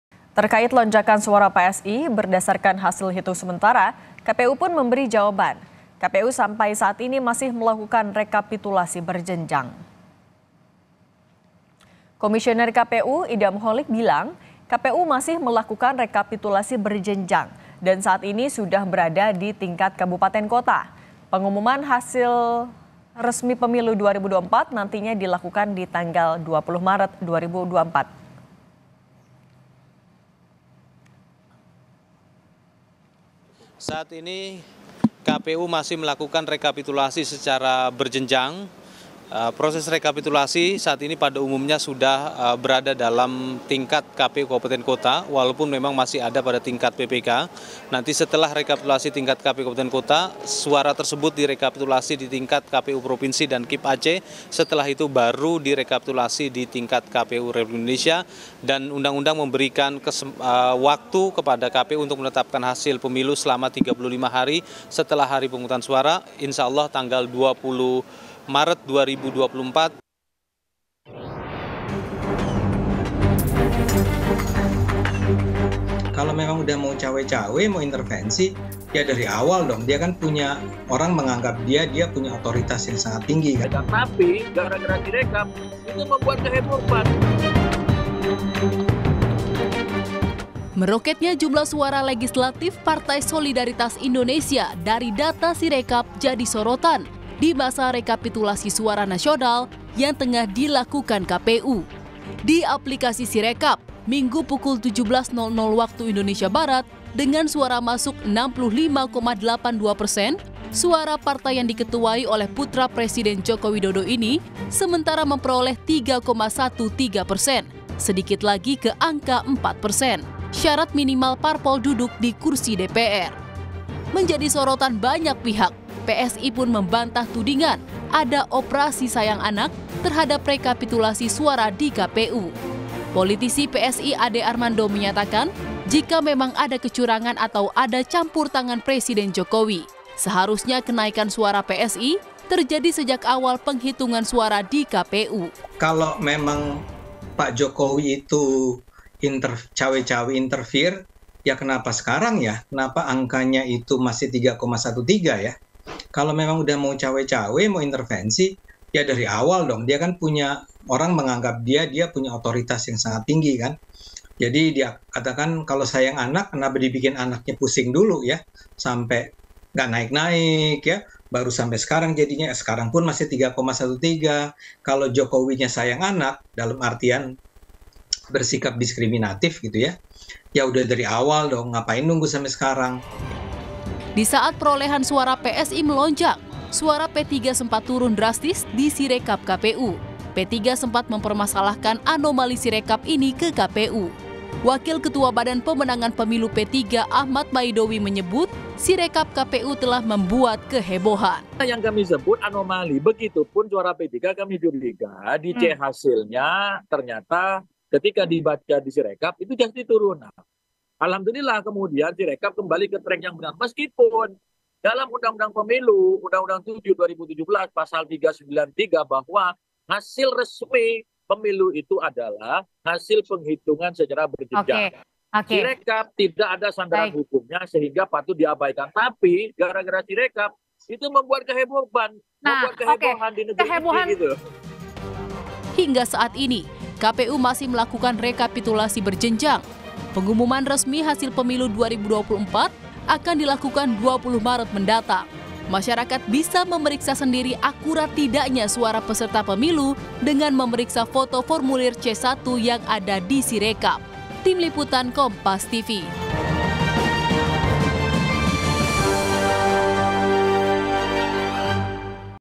Suara PSI Melonjak di Sirekap, KPU: Masih Rekapitulasi Berjenjang
JAKARTA, KOMPAS.TV - Terkait lonjakan suara PSI berdasarkan hasil hitung sementara.

KPU pun memberi jawaban.

Komisioner KPU, Idham Holik bilang, KPU masih melakukan rekapitulasi berjenjang. dan saat ini sudah berada di tingkat kabupaten kota.

Pengumuman resmi hasil pemilu 2024 nantinya di tanggal 20 Maret 2024.